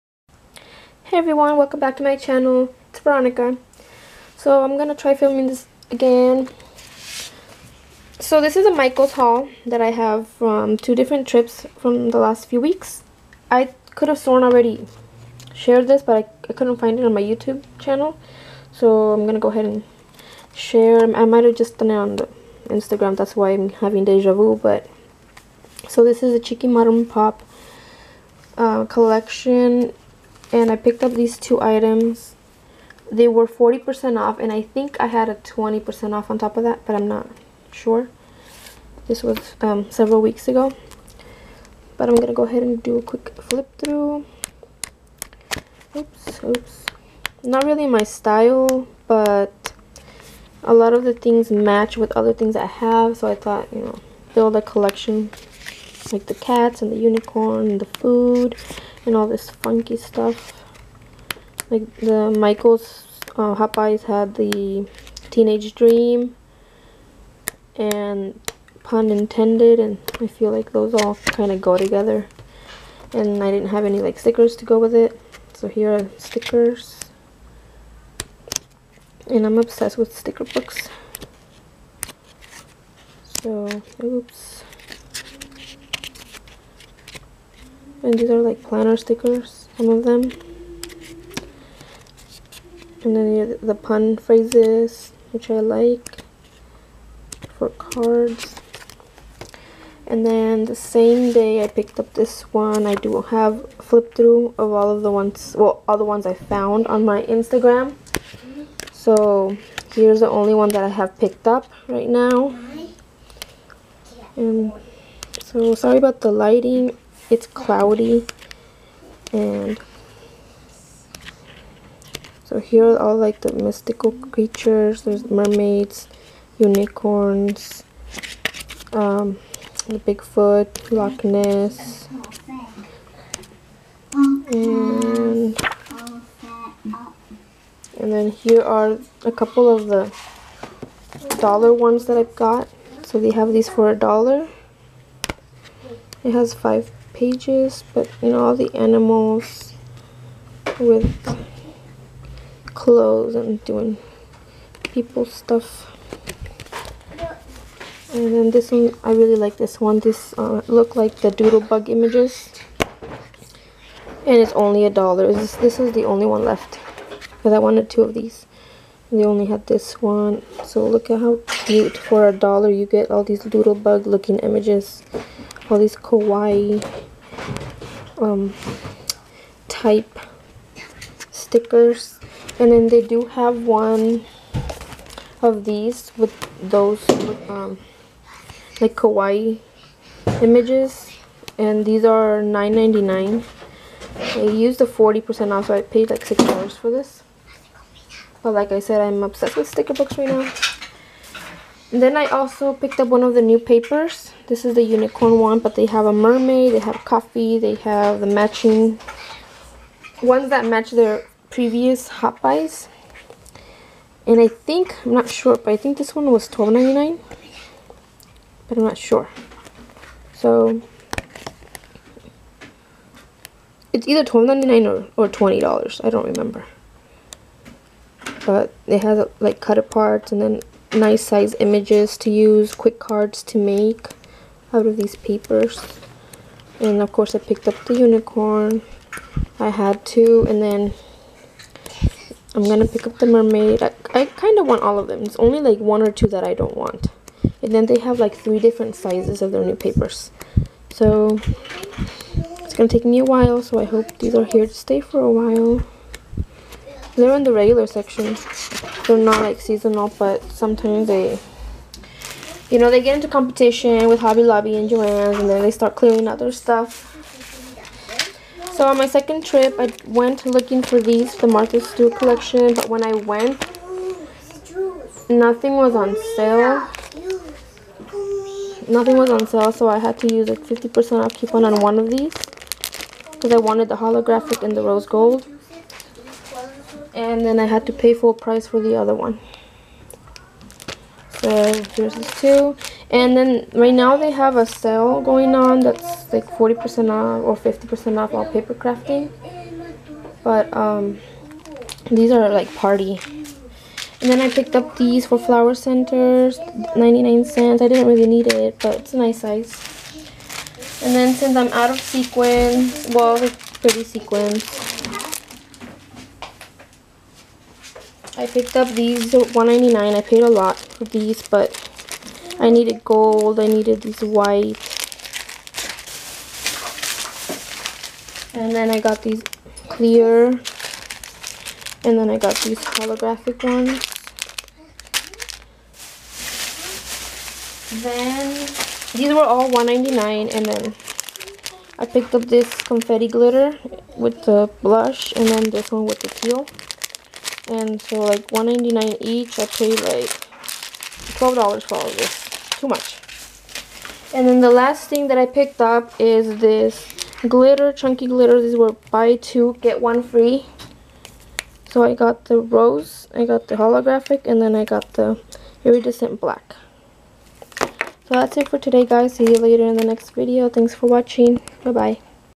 MICHAELS HAUL
Hey guys, this is a haul from about 3 weeks ago, i forgot to post. I thought i filmed it when I did laste Tuesday morning and Hobby Lobby hauls